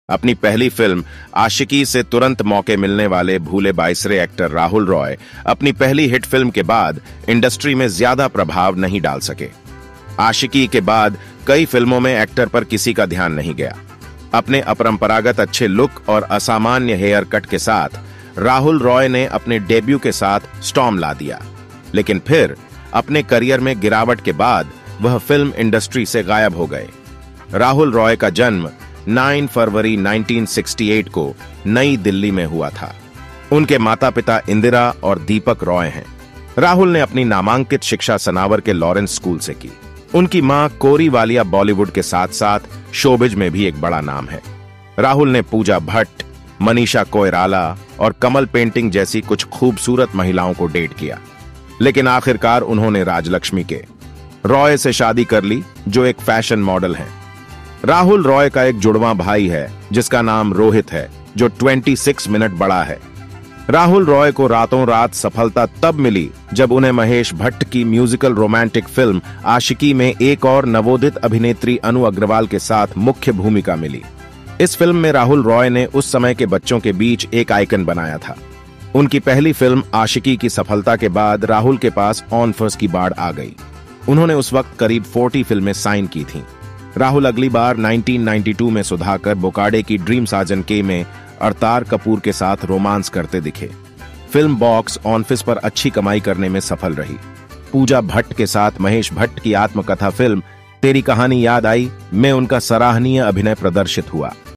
Rahul Roy को आख़िर क्यों छोड़ना पड़ा Bollybood इंडस्ट्री || कहां गायब हो गए राहुल रॉय
Rahul Roy (born 9 February 1968) is an Indian actor, producer and former model known for his works in Hindi films and television.[4][5] Roy began his acting career with the 1990 blockbuster Aashiqui, a Mahesh Bhatt production as the lead actor with newcomer Anu Aggarwal. He then appeared opposite Karishma Kapoor in Sudhakar Bokade's romantic film Sapne Sajan Ke (1992).[6][7] Roy has been honoured with life membership of the International Film And Television Club of Asian Academy of Film & Television.[8][9][10]

Rahul Roy

Rahul Roy in 2012
Born
9 February 1968 (age 55)[1][2]
Bombay, Maharashtra, India[3]
Alma mater
The Lawrence School, Sanawar
Occupation(s)
Actor, model, producer
Years active
1990–present
Spouse
Rajlakshmi Khanvilkar

Roy delivered a notable performance in Mahesh Bhatt's 1993 autobiographical Phir Teri Kahani Yaad Aayee, Roy's character is based on the filmmaker. The film was Zee TV's first mainstream production. Roy then featured in Bhatt's production Jaanam, which was Vikram Bhatt's directorial debut.[11][12][13]

In 2006, Roy participated and won the first season of game show Bigg Boss – the Indian version of Celebrity Big Brother, produced by Endemol India for Colors Viacom 18.[13] Roy has ventured into movie production. His company, Rahul Roy Productions, released its first movie, entitled Elaan, on 25 November 2011 in Bihar. It stars Roy and Rituparna Sen as the main lead.[14